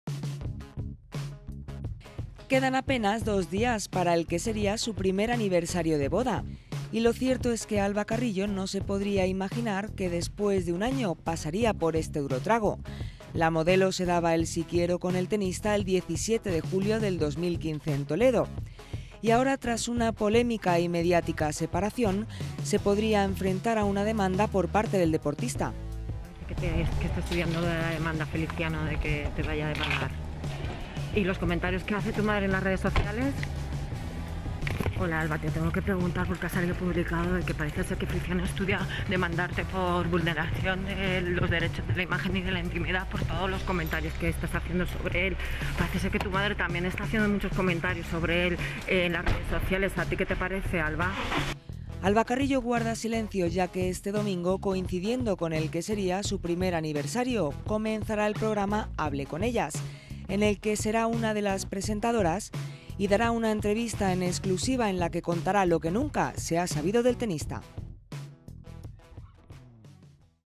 Feliciano López podría demandar a Alba Carrillo
Quedan apenas dos días para el que sería su primer aniversario de boda y lo cierto es que Alba Carrillo no se podría imaginar que después de un año pasaría por este duro trago. La modelo se daba e sí quiero con el tenista el 17 de julio en Toledo, y ahora tras una polémica y mediática separación, se podría enfrentar a una demanda por parte del deportista.